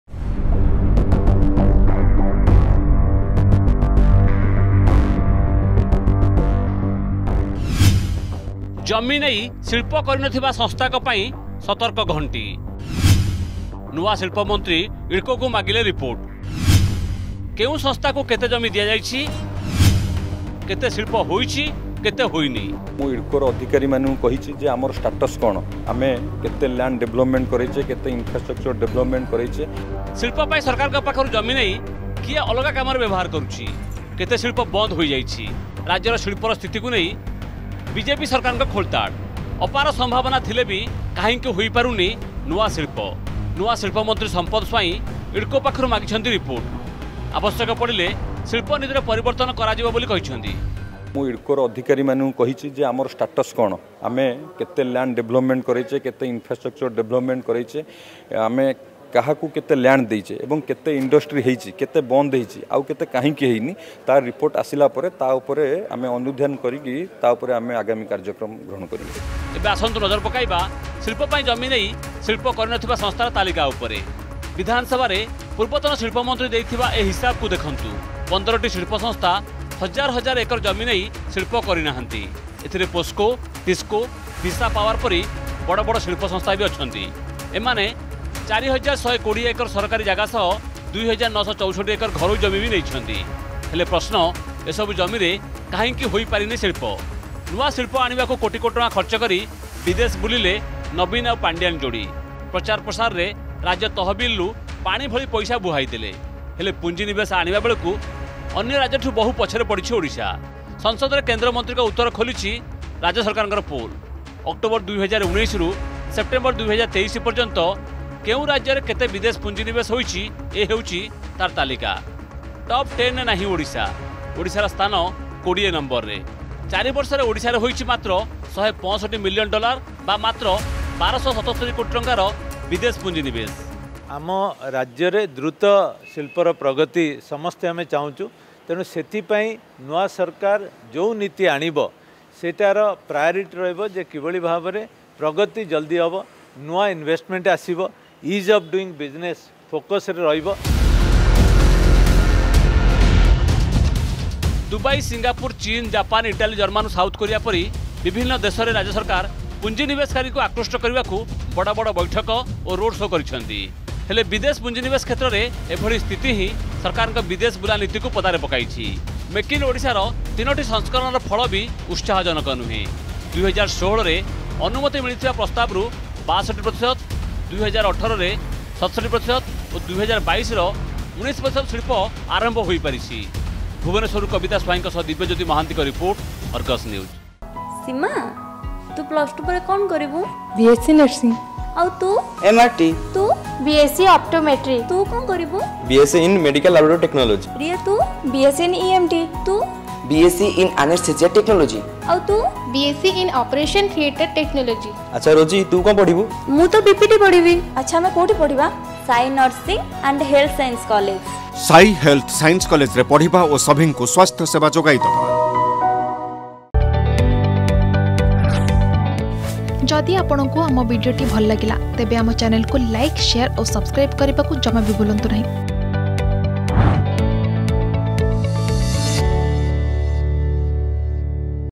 ଜମି ନେଇ ଶିଳ୍ପ ପ୍ରତିଷ୍ଠା ନଥିବା ସଂସ୍ଥାଙ୍କ ପାଇଁ ସତର୍କ ଘଣ୍ଟି || Non-industrial Establishments
ଜମି ନେଇ ଶିଳ୍ପ ପ୍ରତିଷ୍ଠା ନଥିବା ସଂସ୍ଥାଙ୍କ ପାଇଁ ସତର୍କ ଘଣ୍ଟି । ଇଡକୋକୁ ରିପୋର୍ଟ ମାଗିଲେ ନୂଆ ଶିଳ୍ପମନ୍ତ୍ରୀ । ପସ୍କୋ, ଟିକ୍ସୋ,ଭିସା ପାୱାର ଭଳି 15ଟି ଶିଳ୍ପ ସଂସ୍ଥାକୁ ଦିଆଯାଇଛି 7 ହଜାର ଏକରରୁ ଅଧିକ ଜମି । କିନ୍ତୁ ହୋଇନାହିଁ ଶିଳ୍ପ ପ୍ରତିଷ୍ଠା ।